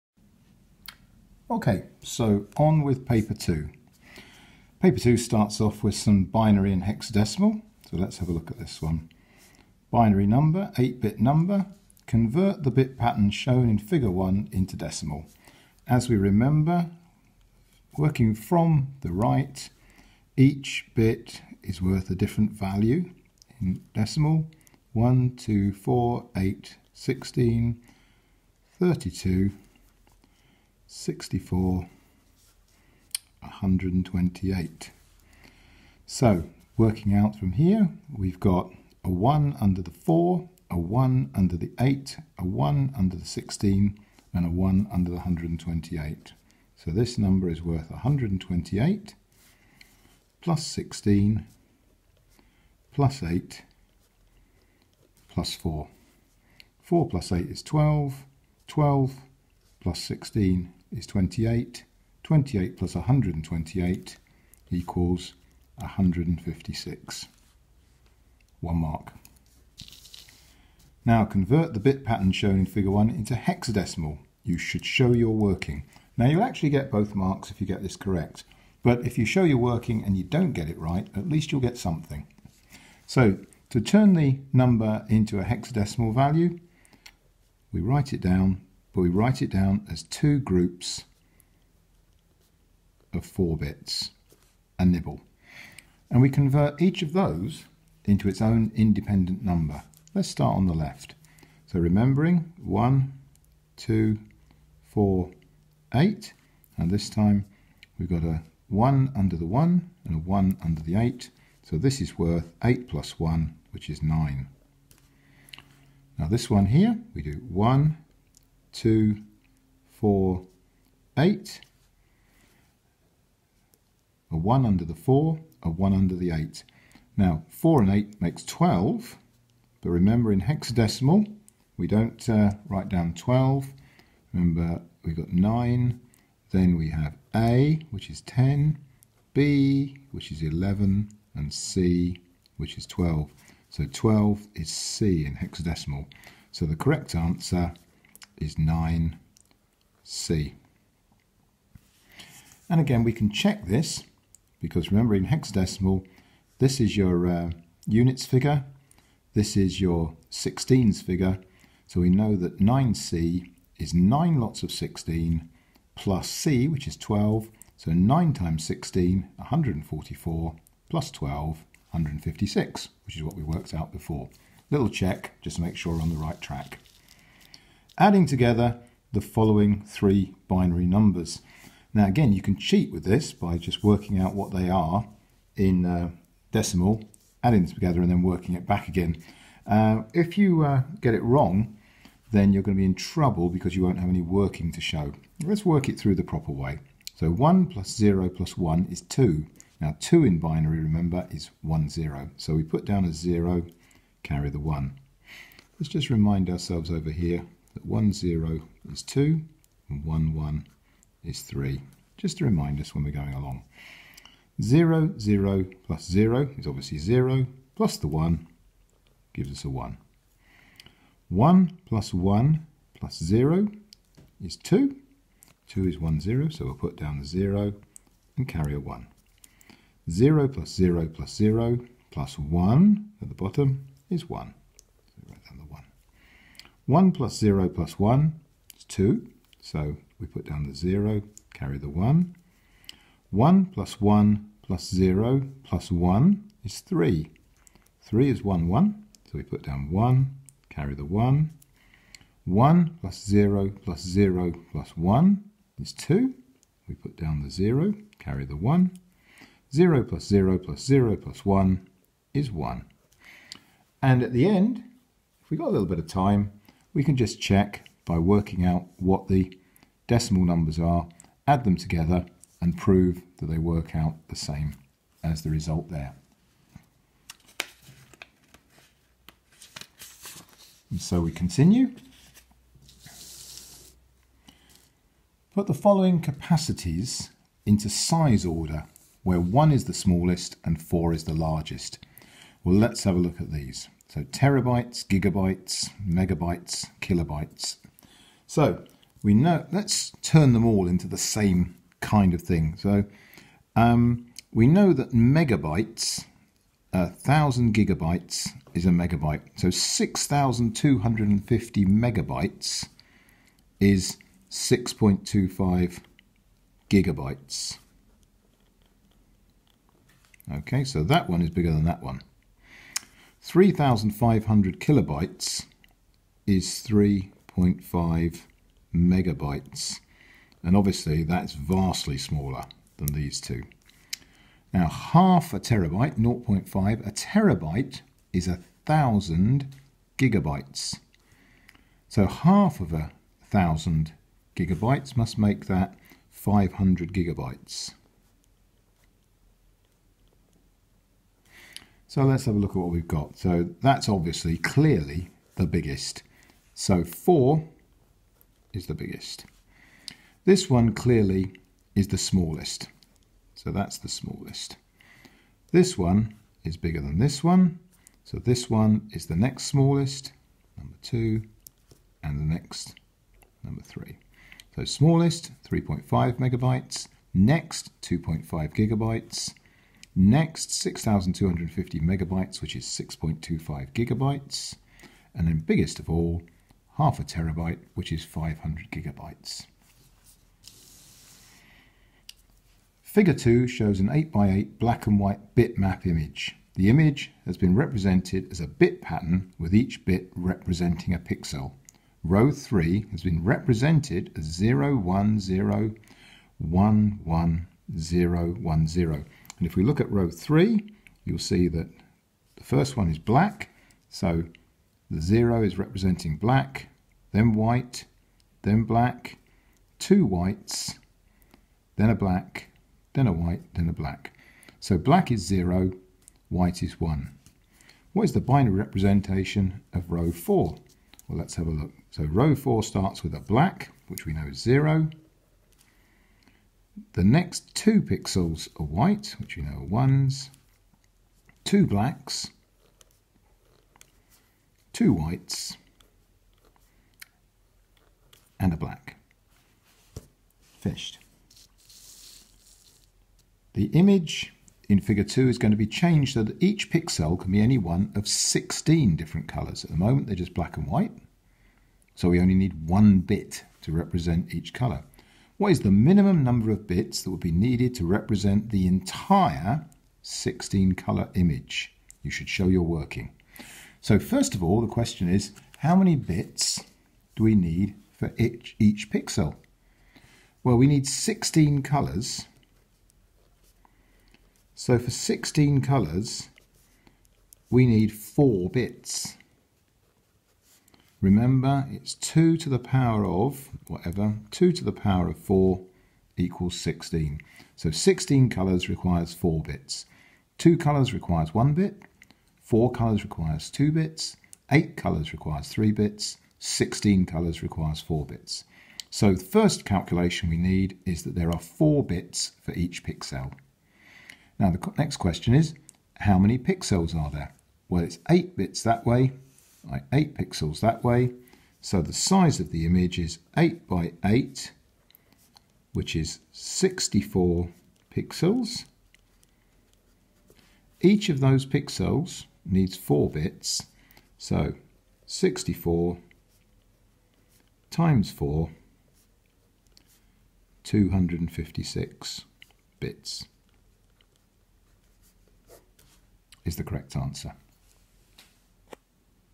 AQA GCSE Computer Science 2018 Paper 2 Questions 1-4
A walk through of the paper. Use this to see how you did in the mock.